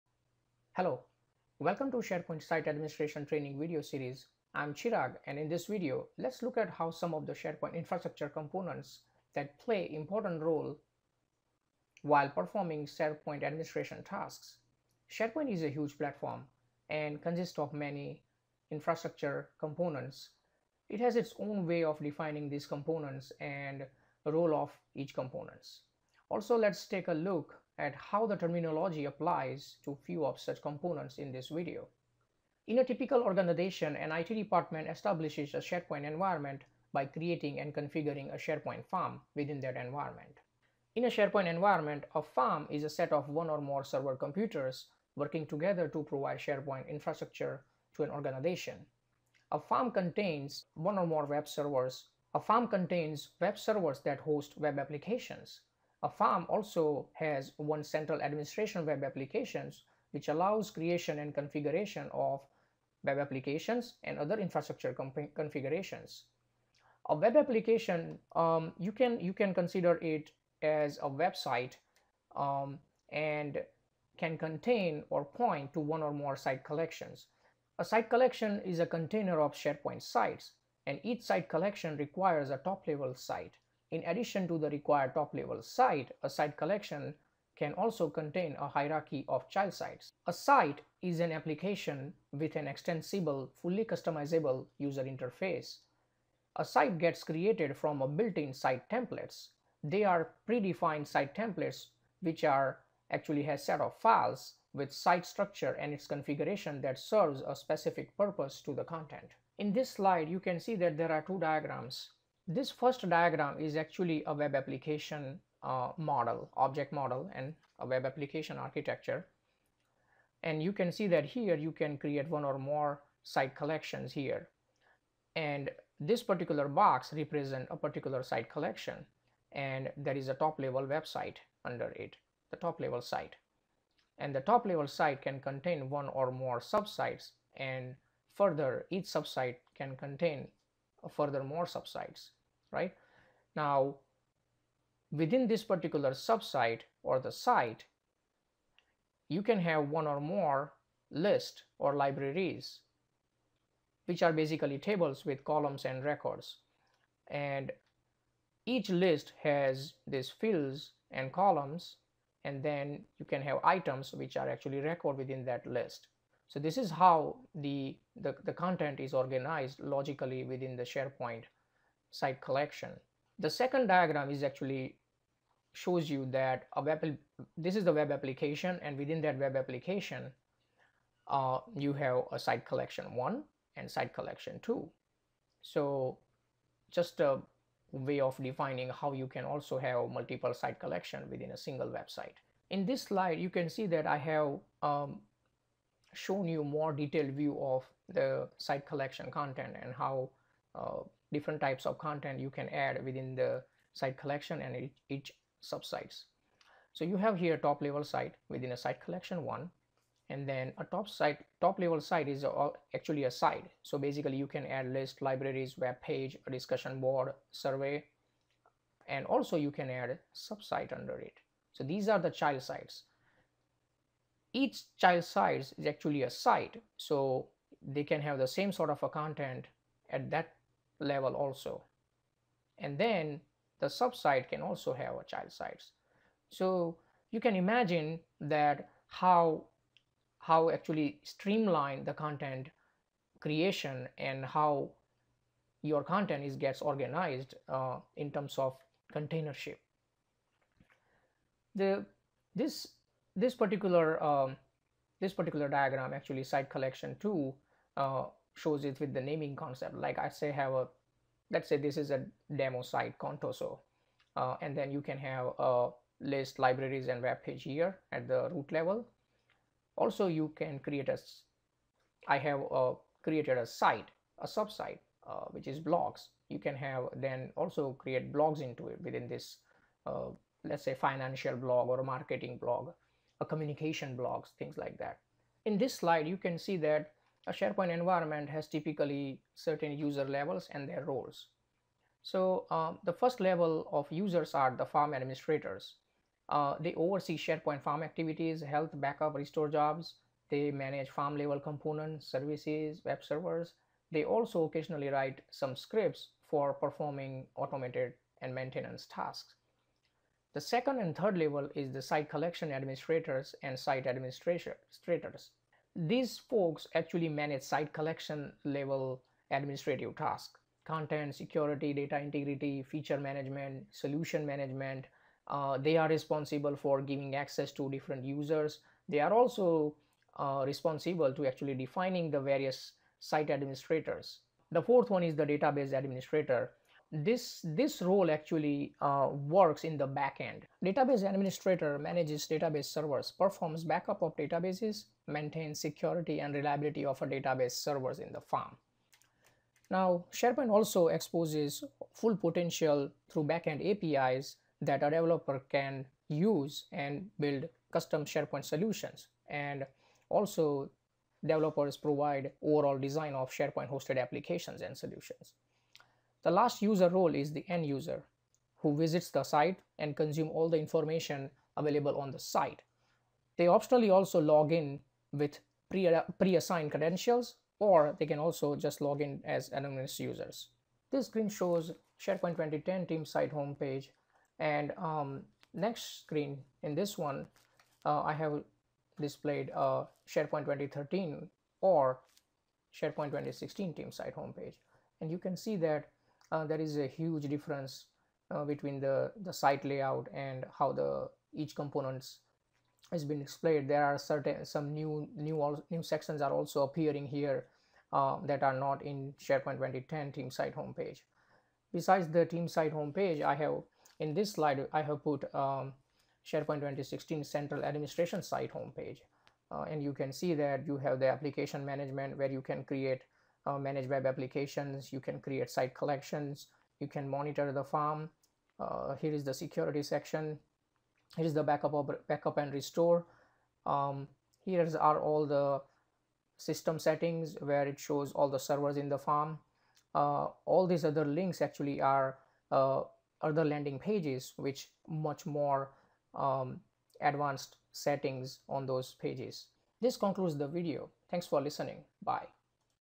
SharePoint 2016 Tutorial - Environment Infrastucture
This video describes SharePoint infrastructure components like Farm, Site Collection, Site etc and how the platform provides means of storing content and content configurations.